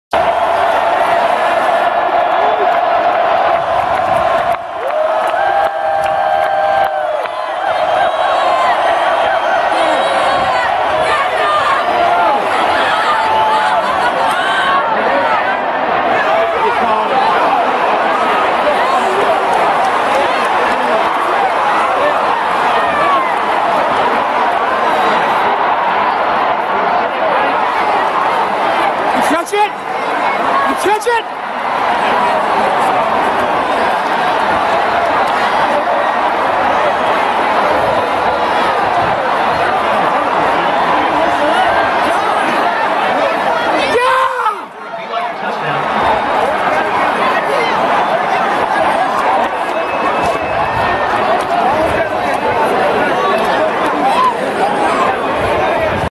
BYU vs Nebraska Hail Mary Catch
Tanner Mangum's hail mary pass to Mitch Matthews to defeat Nebraska